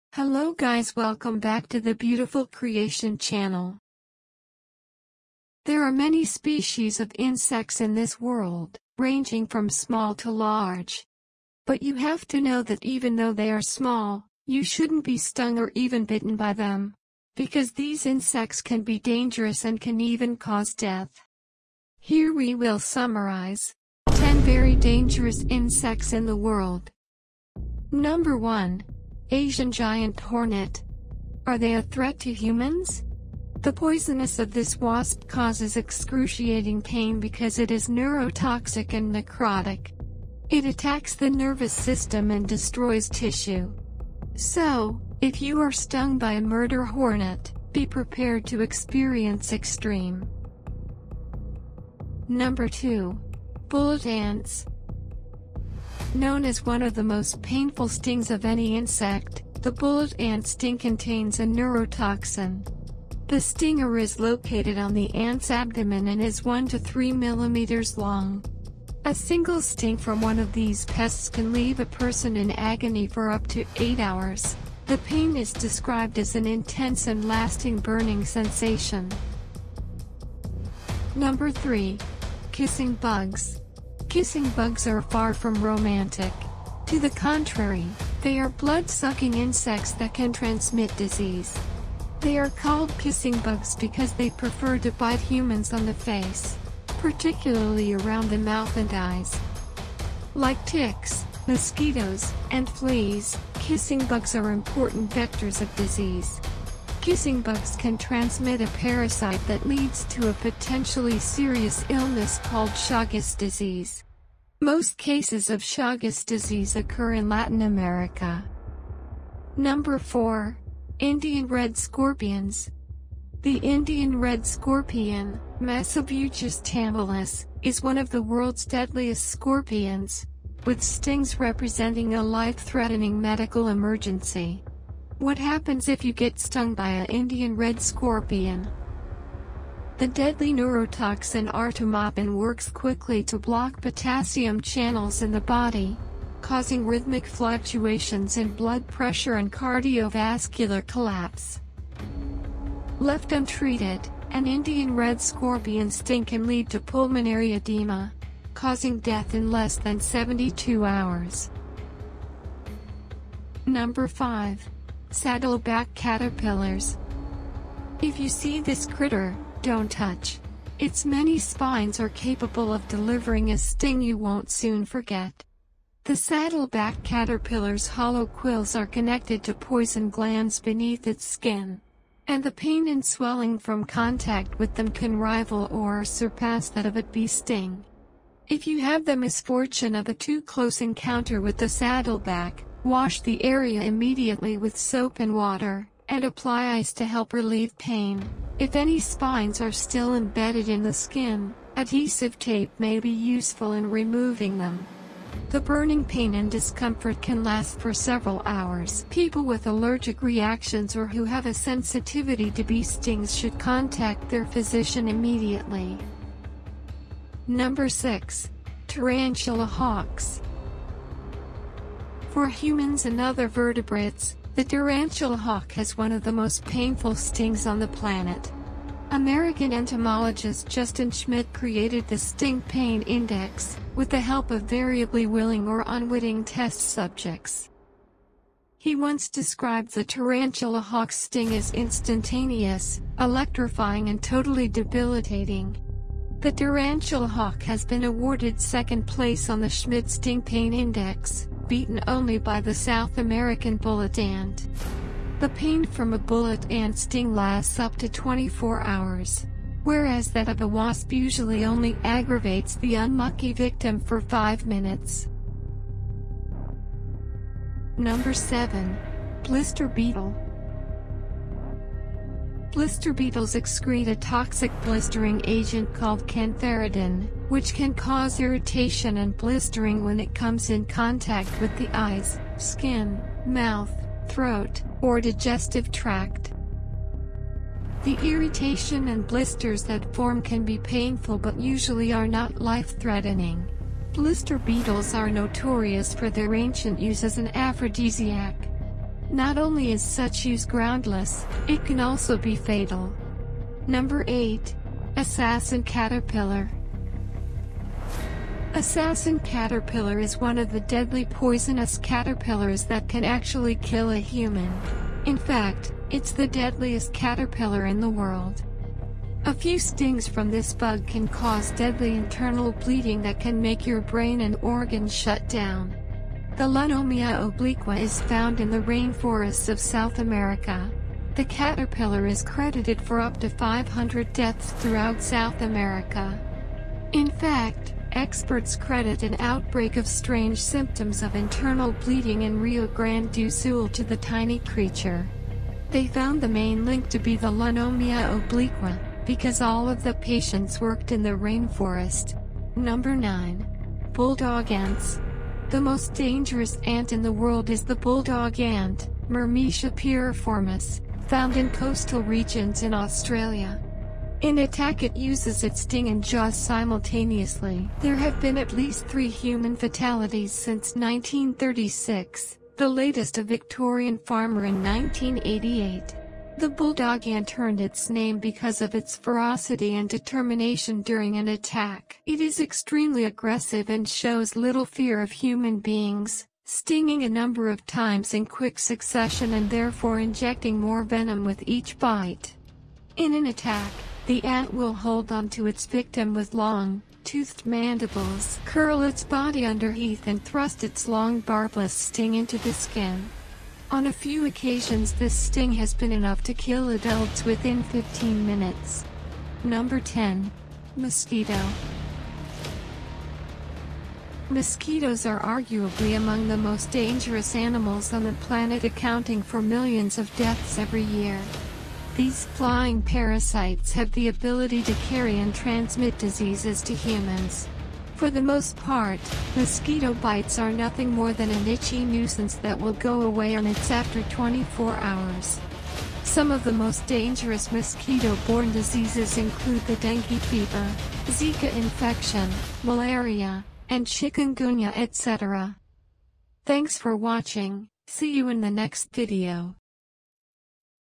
TOP 10 Dangerous Insects In The World | The Most dangerous Insects In The World
In this video I summarize 10 very dangerous insects in the world, if you see these insects, don't try to hold them, and don't get stung. Because among them there are insects that can kill you.